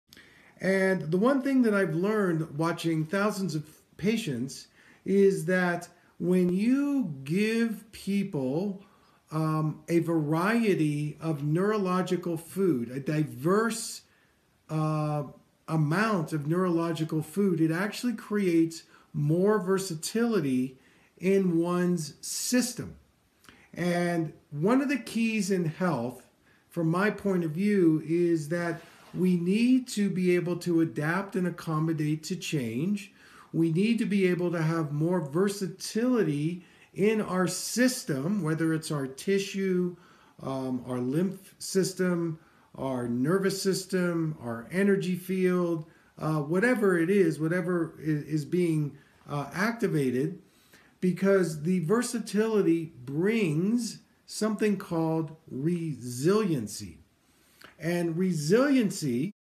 Neurological food
We need to adapt to change and increase our versatility within our internal systems - more neurological foods can help!